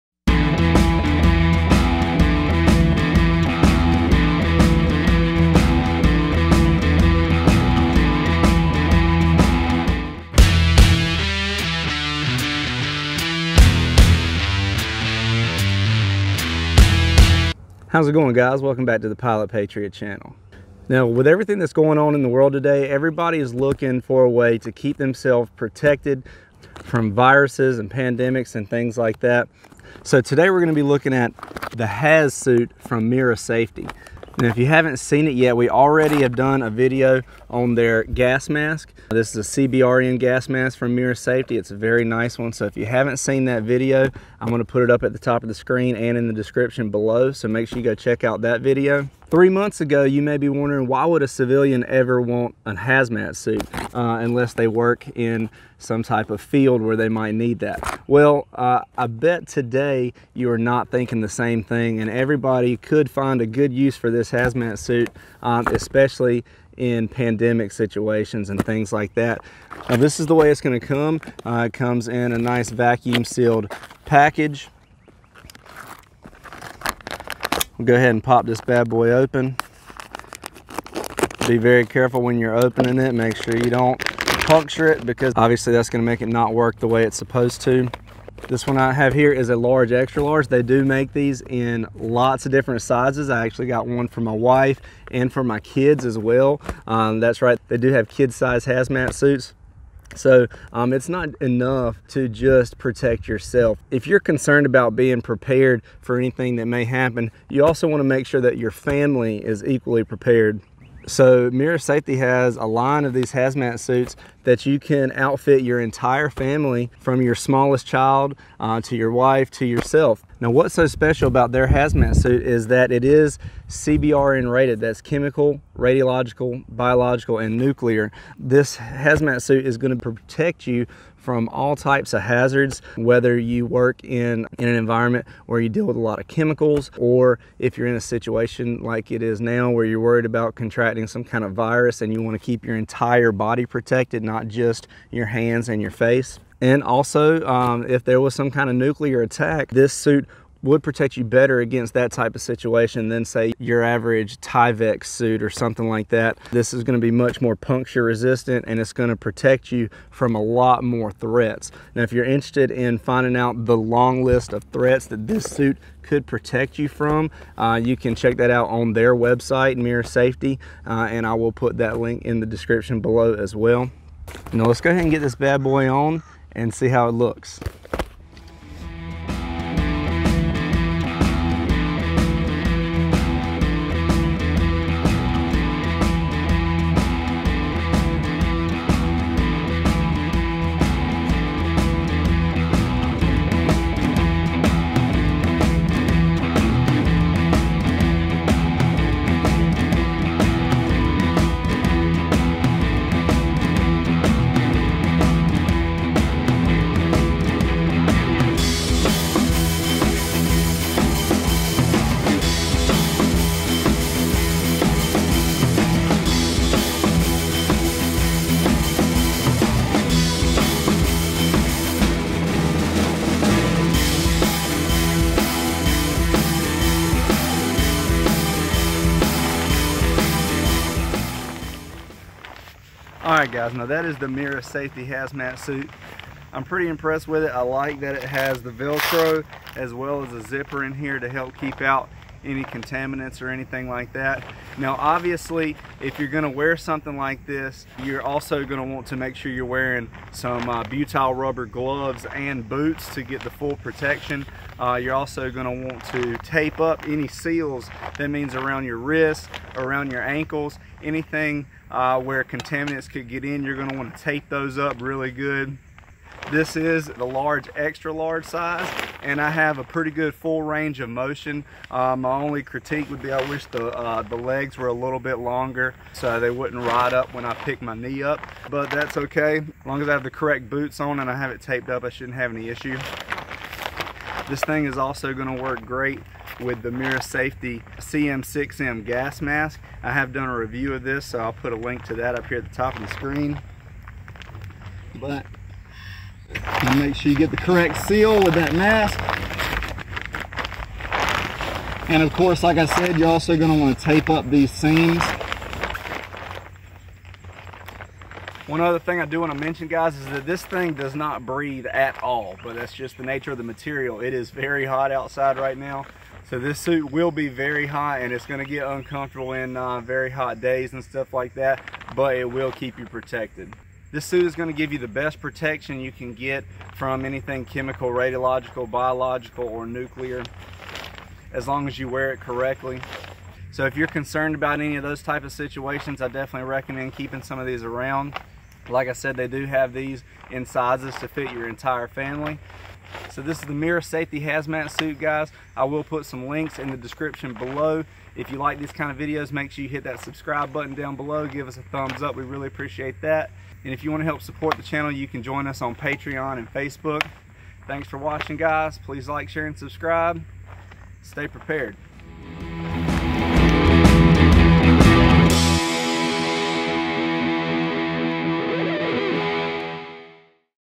CBRN Hazmat Suit / MIRA Safety
If you want the ultimate CBRN protection for your entire body, a Hazmat suit from MIRA Safety might just be what you need. Full body protection from chemicals, viruses, pandemic, even nuclear fallout.

Butyl Rubber Gloves
Protective Rubber Overboots
Tyrosafe Potassium Iodide
IOSAT Potassium Iodide